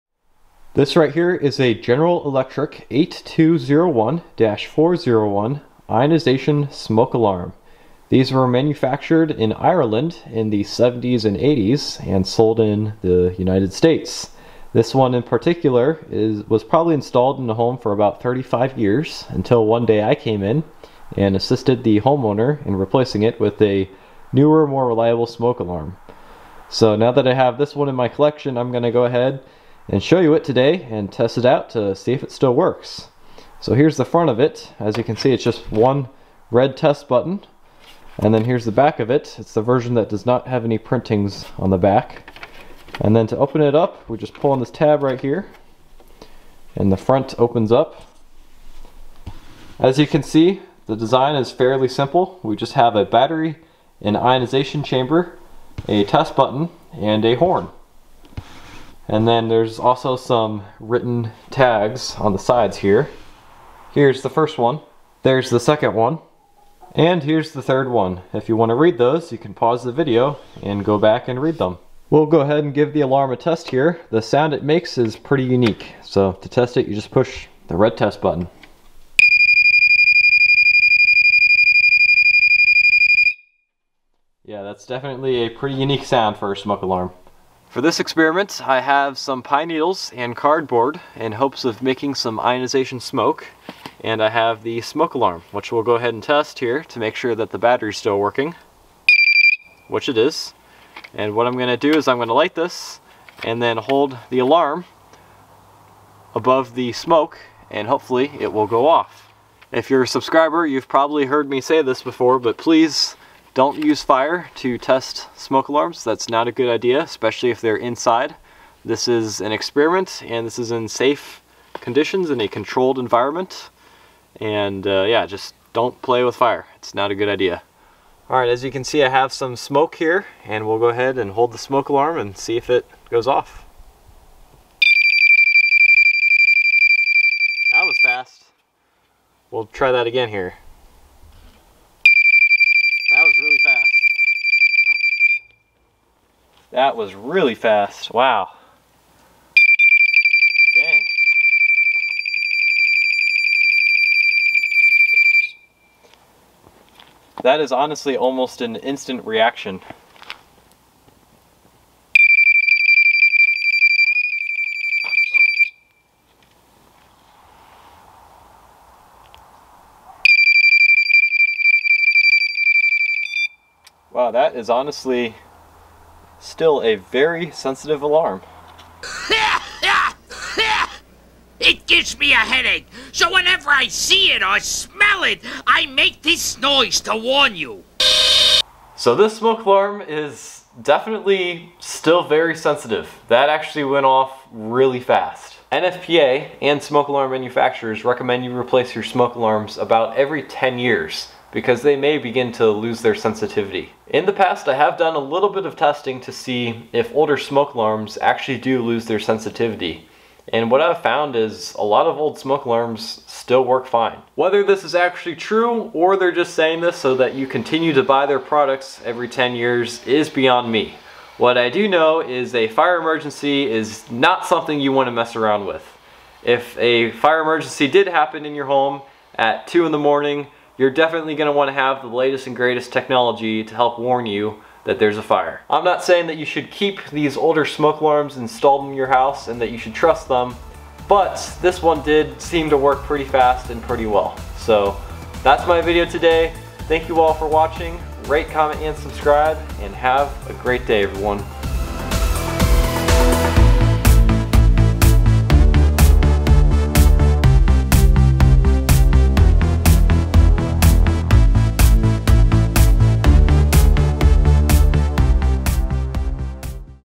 Testing the Sensitivity of a 43-Year-Old Smoke Alarm | Impressive Results!
I tested a General Electric model 8201-401 ionization smoke alarm with real fire and smoke to see if it has lost smoke sensitivity over the years. Despite it being 43 years old the alarm still had an impressive reaction time.